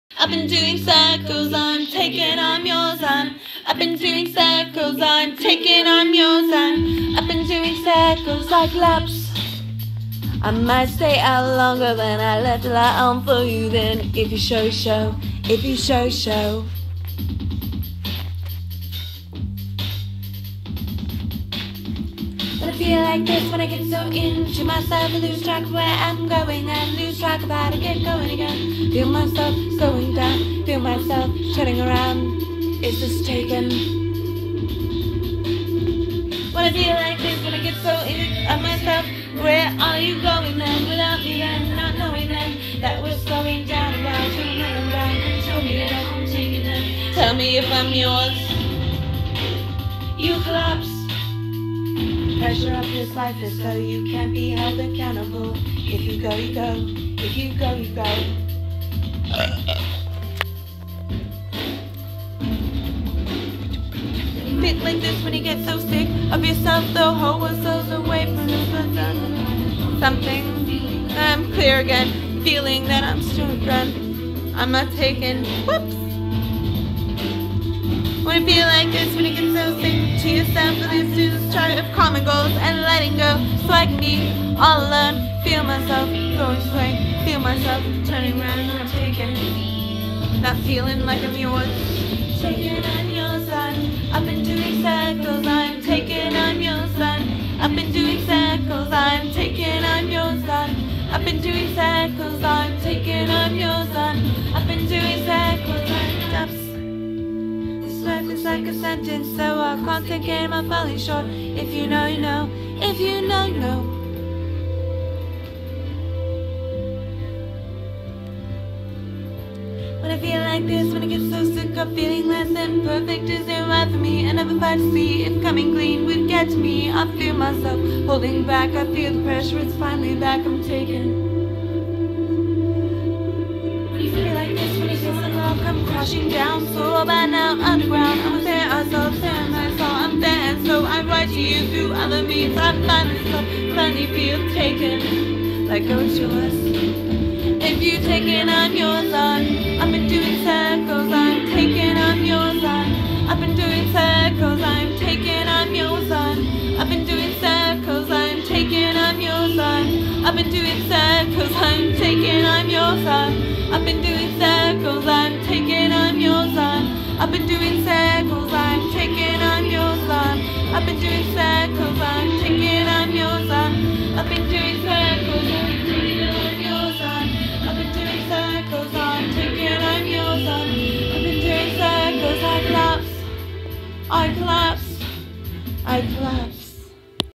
Drunk t&s songtime
Totttttally forgot words a few times,Like karaoke